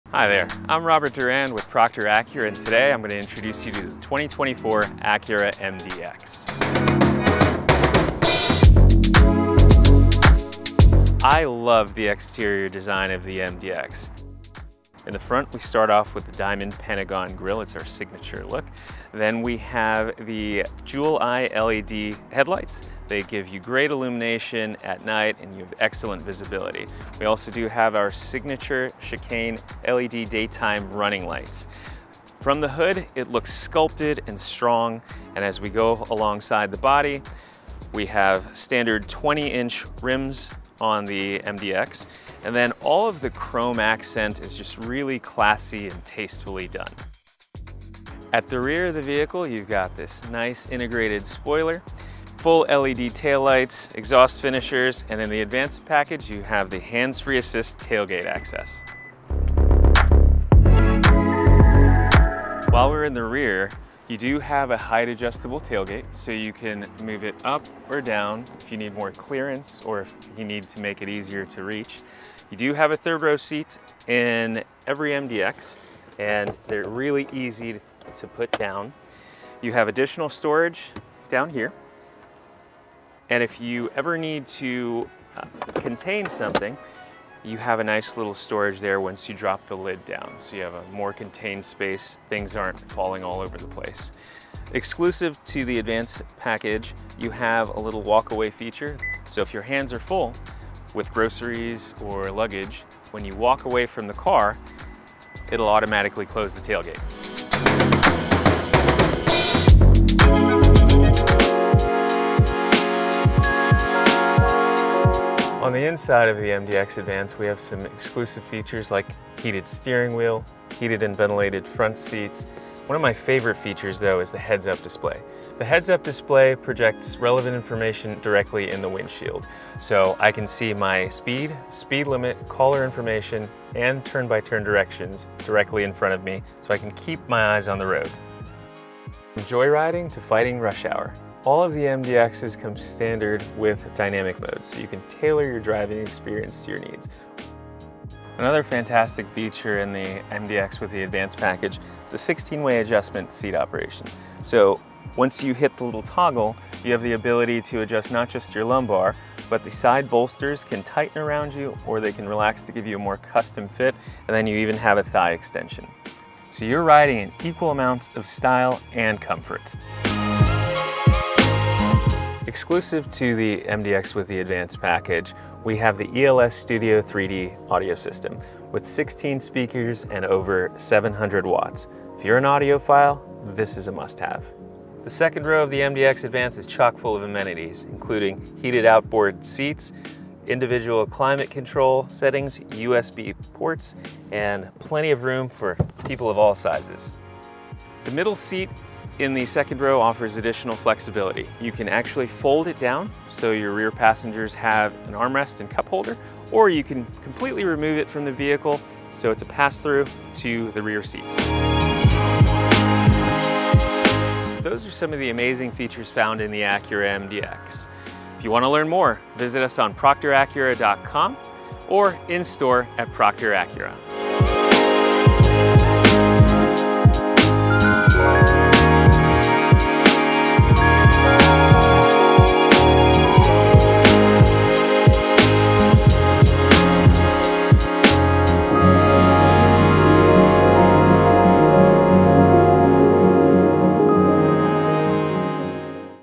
2024 Acura MDX Features Walkaround | Proctor Acura
Learn more about the new 2024 Acura MDX as Robert Durand of Proctor Acura shows us some of the most notable features!
High-end craftmanship extends to every row with seating for up to 7, and a removable 2nd row center seat. You're ready for any adventure with standard safety features and performance technology.
Visit us in store, or online at ProctorAcura.com